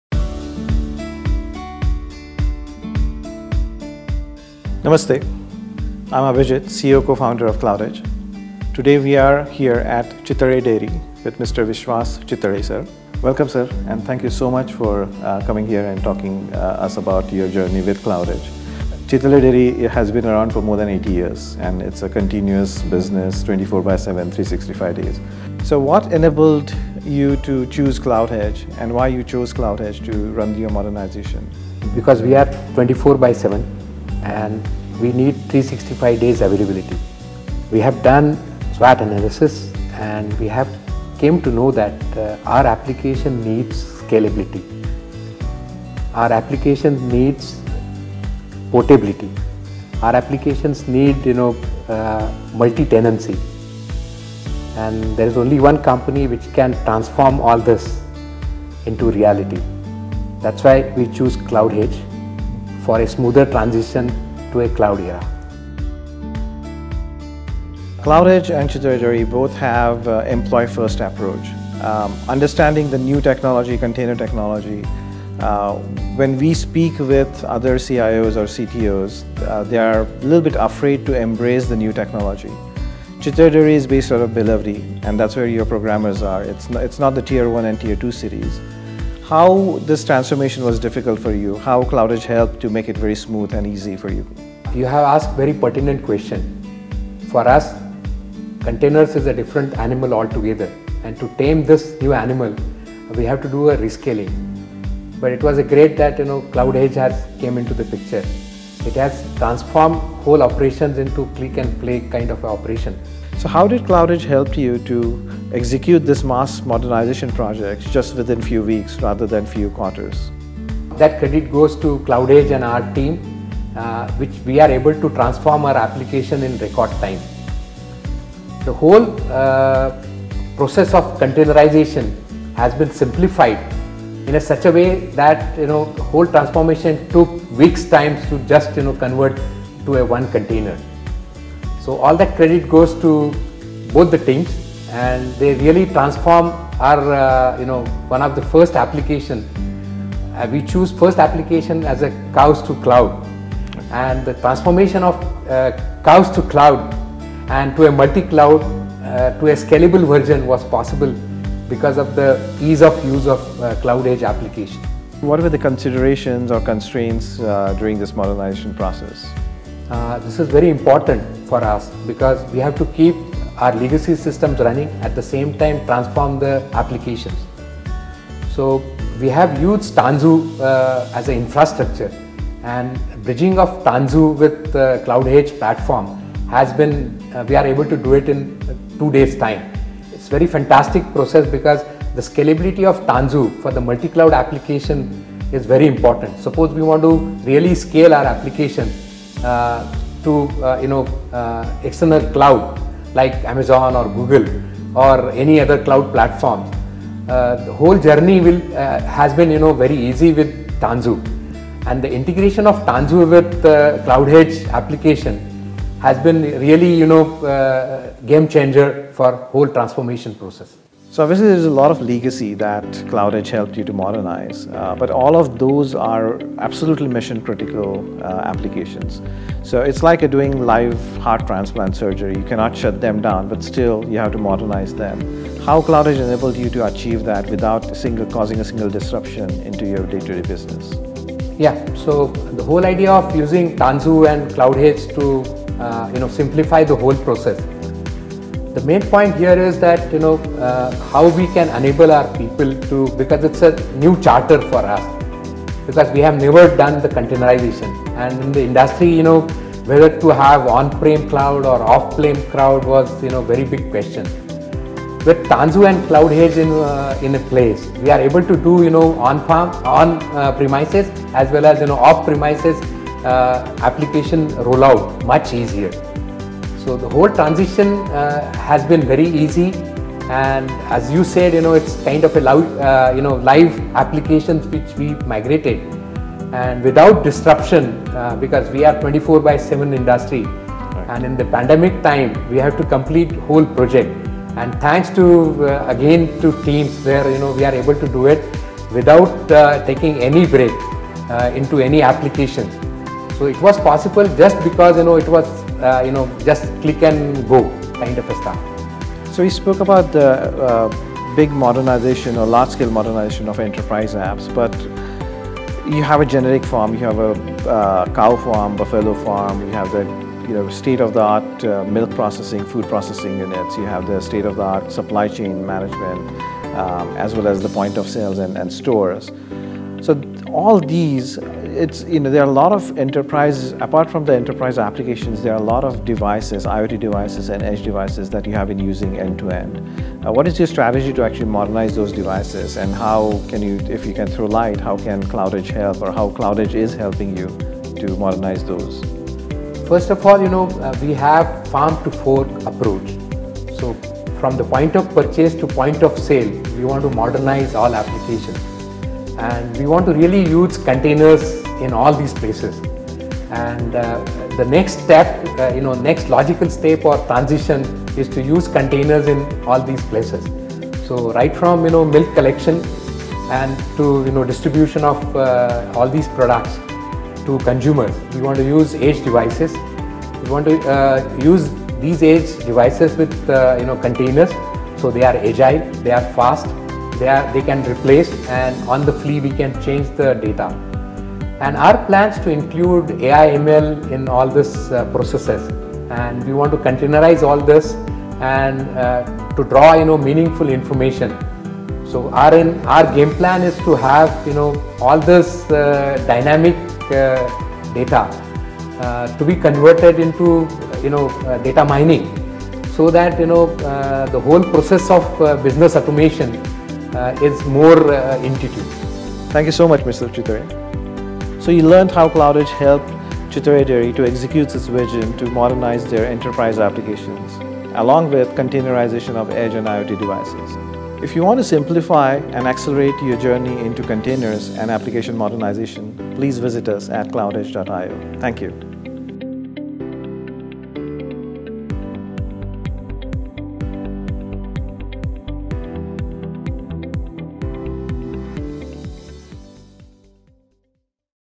CloudHedge's Automated App Modernization platform accelerates Chitale's journey from Farm to Fridge.
Vishwas Chitale - CEO & CTO Chitale Dairy, shares his story on how CloudHedge's automated App Modernization Platform accelerated the modernization of Chitale's large-scale enterprise applications and containerized EDGE and IoT devices.